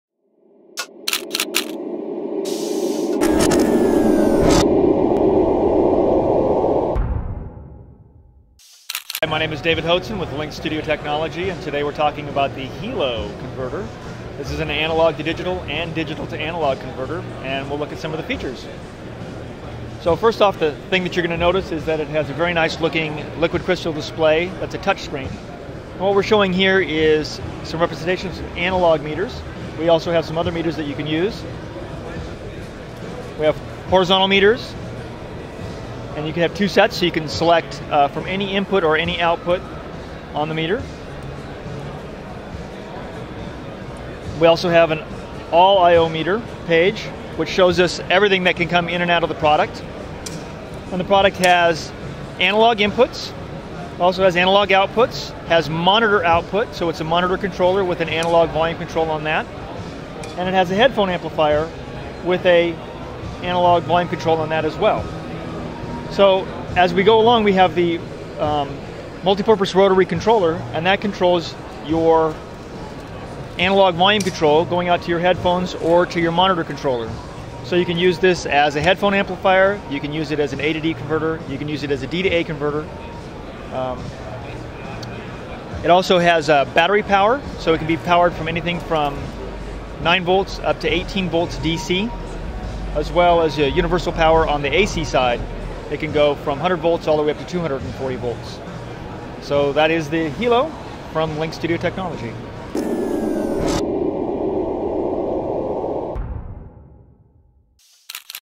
Lynx Studio Technology Hilo Digital Converter - Video Demo AES 2011
At AES 2011 in New York, Lynx unveiled its new Hilo high resolution digital A/D D/A digital audio converter system in this video demo for George Petersen of HDVideoPro/CineSoundPro magazine.